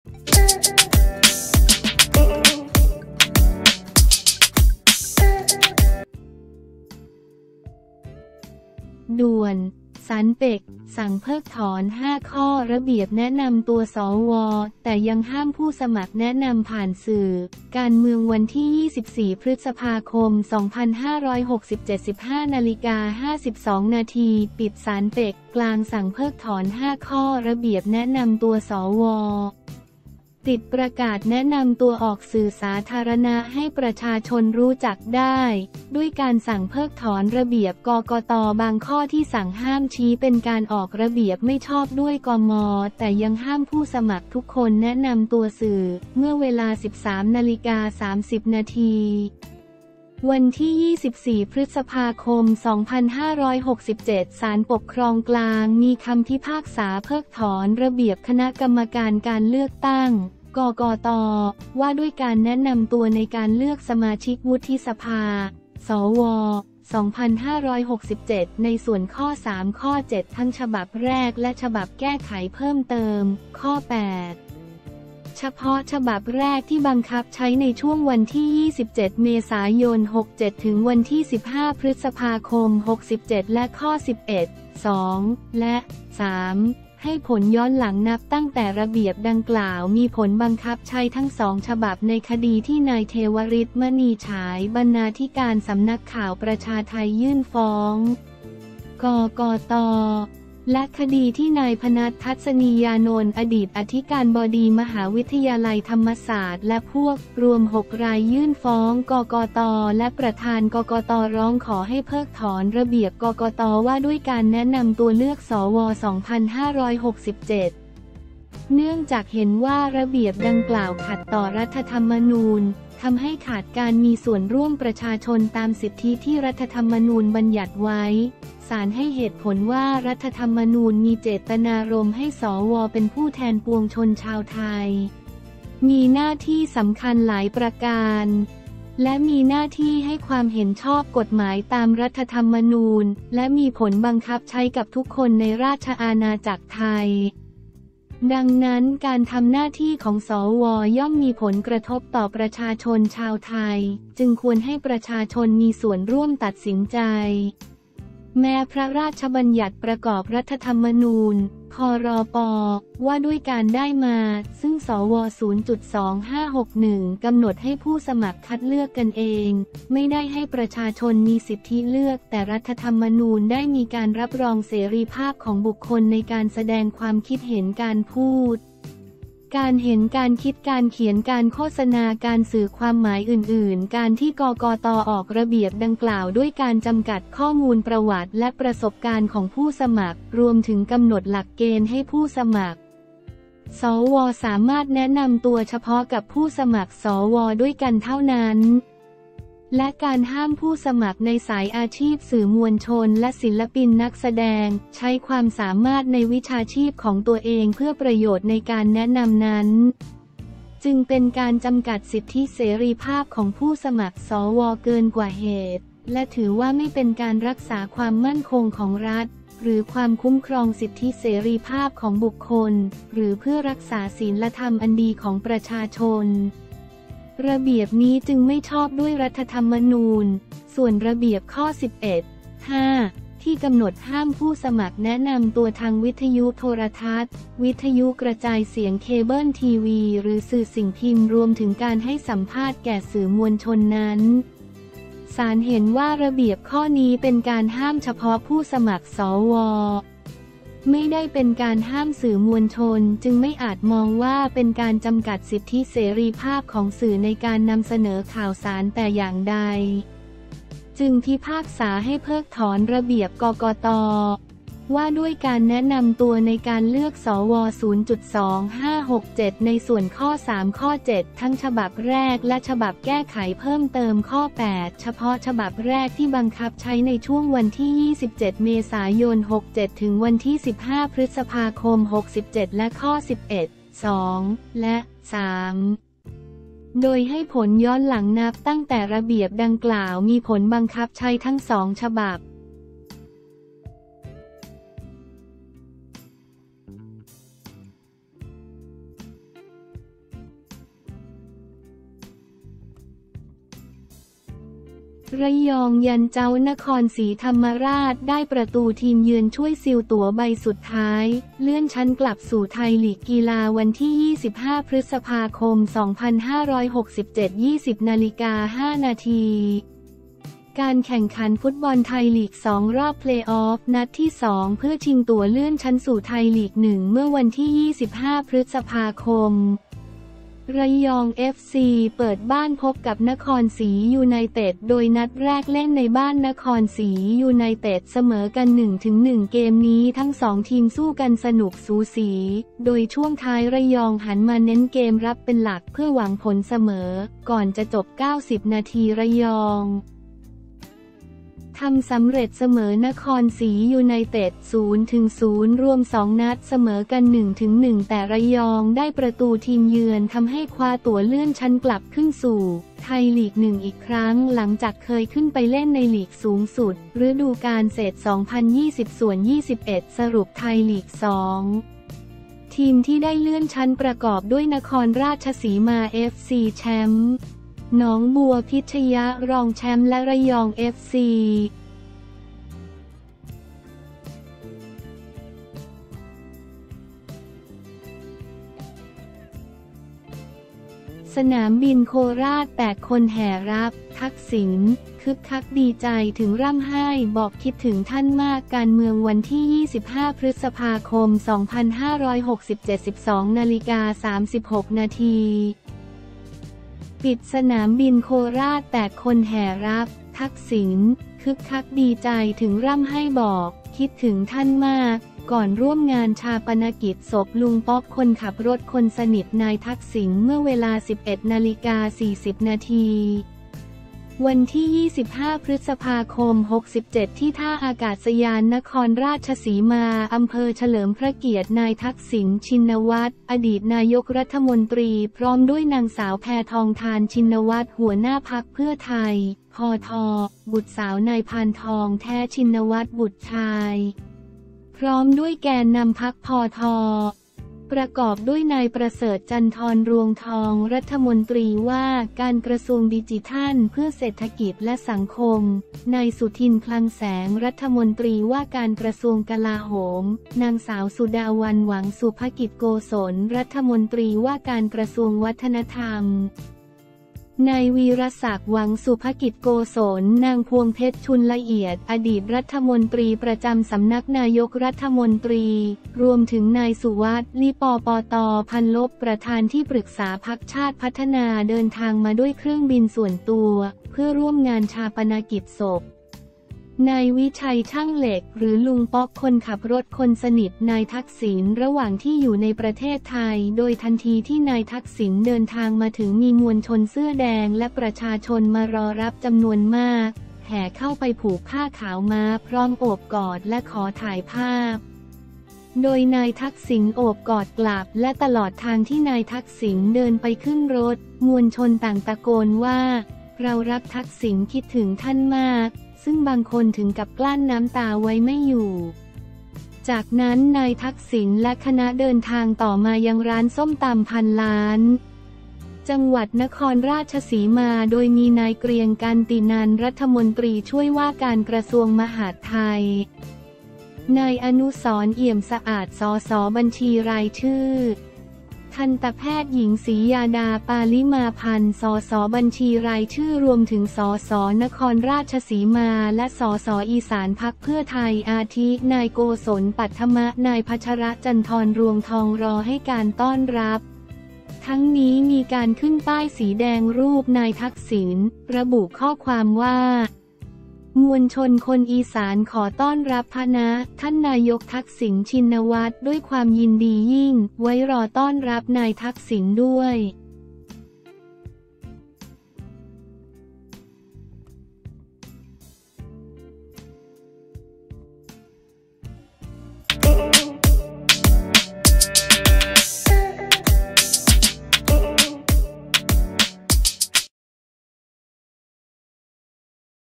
ด่วน! ศาลปค สั่งเพิกถอน 5 ข้อระเบียบแนะนำตัว สว แต่ยังห้ามผู้สมัครแนะนำผ่านสื่อ
24 พ.ค. 2567
15:52 น.
ปิด
ศาลปค.กลาง สั่งเพิกถอน 5 ข้อระเบียบแนะนำตัวสว. -ติดประกาศแนะนำตัวออกสื่อสาธารณะให้ประชาชนรู้จักได้ ด้วยการสั่งเพิกถอนระเบียบ กกต.บางข้อที่สั่งห้าม ชี้เป็นการออกระเบียบไม่ชอบด้วยกม. แต่ยังห้ามผู้สมัครทุกคนแนะนำตัวสื่อ

เมื่อเวลา 13.30 น. วันที่ 24 พ.ค.2567 ศาลปกครองกลาง มีคำพิพากษาเพิกถอนระเบียบคณะกรรมการการเลือกตั้ง (กกต.) ว่าด้วยการแนะนำตัวในการเลือกสมาชิกวุฒิสภา (สว.) 2567 ในส่วนข้อ 3 ข้อ 7 ทั้งฉบับแรกและฉบับแก้ไขเพิ่มเติม ข้อ 8 เฉพาะฉบับแรกที่บังคับใช้ในช่วง 27 เม.ย.67-15 พ.ค.67 และข้อ 11 (2) และ (3) ให้ผลย้อนหลังนับตั้งแต่ระเบียบดังกล่าวมีผลบังคับใช้ทั้ง 2 ฉบับ

ในคดีที่ นายเทวฤทธิ์ มณีฉาย บรรณาธิการสำนักข่าวประชาไท ยื่นฟ้อง กกต. และคดีที่ นายพนัส ทัศนียานนท์ อดีตอธิการบดีมหาวิทยาลัยธรรมศาสตร์และพวก รวม 6 ราย ยื่นฟ้อง กกต.และประธาน กกต.ร้องขอให้เพิกถอนระเบียบ กกต.ว่าด้วยการแนะนำตัวเลือก สว.2567 เนื่องจากเห็นว่าระเบียบดังกล่าวขัดต่อรัฐธรรมนูญ ทำให้ขาดการมีส่วนร่วมประชาชนตามสิทธิที่รัฐธรรมนูญบัญญัติไว้

ศาลให้เหตุผลว่า รัฐธรรมนูญมีเจตนารมณ์ให้ สว.เป็นผู้แทนปวงชนชาวไทย มีหน้าที่สำคัญหลายประการ และมีหน้าที่ให้ความเห็นชอบกฎหมายตามรัฐธรรมนูญ และมีผลบังคับใช้กับทุกคนในราชอาณาจักรไทย ดังนั้น การทำหน้าที่ของ สว.ย่อมมีผลกระทบต่อประชาชนชาวไทย จึงควรให้ประชาชนมีส่วนร่วมตัดสินใจ

แม้พระราชบัญญัติประกอบรัฐธรรมนูญ (พ.ร.ป.) ว่าด้วยการได้มาซึ่ง สว.2561 กำหนดให้ผู้สมัครคัดเลือกกันเอง ไม่ได้ให้ประชาชนมีสิทธิเลือก แต่รัฐธรรมนูญได้มีการรับรองเสรีภาพของบุคคลในการแสดงความคิดเห็นการพูด การเห็น การคิด การเขียน การโฆษณา การสื่อความหมายอื่นๆ
การที่ กกต.ออกระเบียบดังกล่าว ด้วยการจำกัดข้อมูลประวัติและประสบการณ์ของผู้สมัคร รวมถึงกำหนดหลักเกณฑ์ให้ผู้สมัคร สว.สามารถแนะนำตัวเฉพาะกับผู้สมัครสว.ด้วยกันเท่านั้น และการห้ามผู้สมัครในสายอาชีพสื่อมวลชนและศิลปินนักแสดง ใช้ความสามารถในวิชาชีพของตัวเองเพื่อประโยชน์ในการแนะนำนั้น จึงเป็นการจำกัดสิทธิเสรีภาพของผู้สมัครสว.เกินกว่าเหตุ และถือว่าไม่เป็นการรักษาความมั่นคงของรัฐ หรือความคุ้มครองสิทธิเสรีภาพของบุคคล หรือเพื่อรักษาศีลธรรมอันดีของประชาชน ระเบียบนี้จึงไม่ชอบด้วยรัฐธรรมนูญ

ส่วนระเบียบข้อ 11 (5) ที่กำหนดห้ามผู้สมัครแนะนำตัวทางวิทยุโทรทัศน์ วิทยุกระจายเสียง เคเบิลทีวี หรือสื่อสิ่งพิมพ์ รวมถึงการให้สัมภาษณ์แก่สื่อมวลชนนั้น ศาลเห็นว่าระเบียบข้อนี้เป็นการห้ามเฉพาะผู้สมัคร สว. ไม่ได้เป็นการห้ามสื่อมวลชน จึงไม่อาจมองว่าเป็นการจำกัดสิทธิเสรีภาพของสื่อในการนำเสนอข่าวสารแต่อย่างใด

จึงพิพากษาให้เพิกถอนระเบียบกกต. ว่าด้วยการแนะนำตัวในการเลือก สว.2567 ในส่วนข้อ 3 ข้อ 7 ทั้งฉบับแรก และฉบับแก้ไขเพิ่มเติม ข้อ 8 เฉพาะฉบับแรกที่บังคับใช้ในช่วง 27เม.ย.67-15 พ.ค.67 และข้อ 11 (2) และ (3) โดยให้ผลย้อนหลังนับตั้งแต่ระเบียบดังกล่าวมีผลบังคับใช้ทั้งสองฉบับ